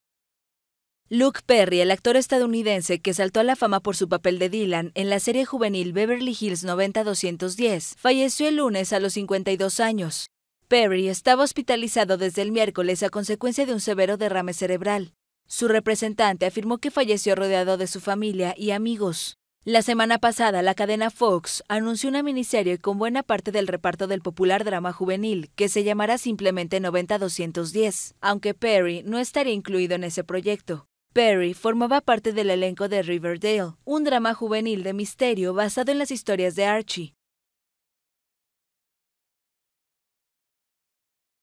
Muere Luke Pery, Dylan en"Beverly Hills, 90210"
El actor estadounidense Luke Perry, famoso por su papel de Dylan McKay en la serie de los 90 "Beverly Hills 90210", falleció el lunes a los 52 años por un severo derrame cerebral.